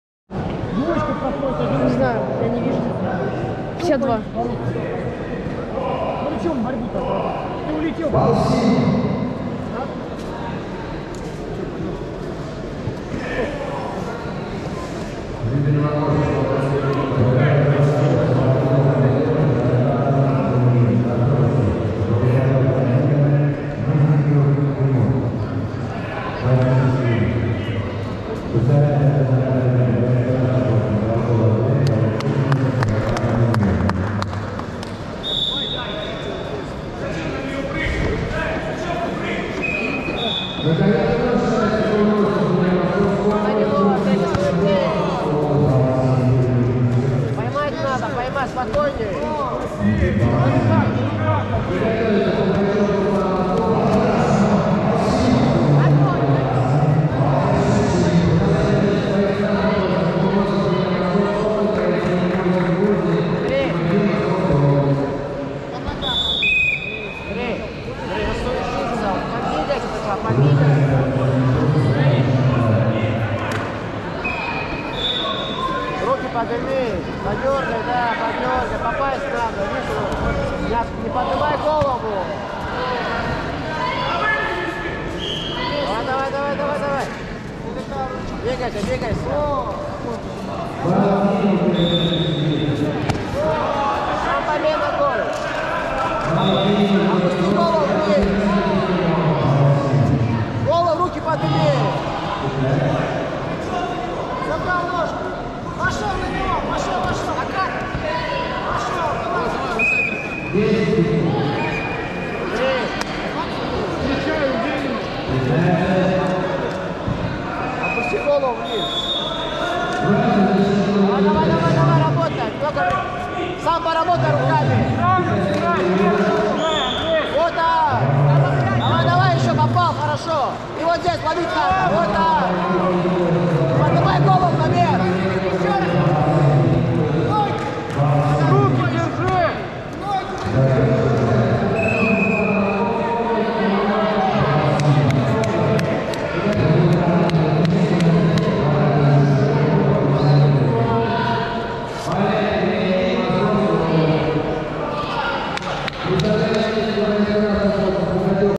Долгов Андрей (красный пояс), клуб ЯРОПОЛК, Всероссийский турнир по Рукопашному Бою, гТула, 22.10.22
С 21 по 22 октября 2022 года в г. Тула прошёл Всероссийский турнир по рукопашному бою среди юношей и девушек от 12 до 17 лет.
🥉 Поверенков Денис- 3 место

2. Клуб в яндекс ДЗЕН
5. Резервный канал клуба в ЮТУБЕ (бои спортсменов клуба и т.д.- проф. и любительская видео съёмка)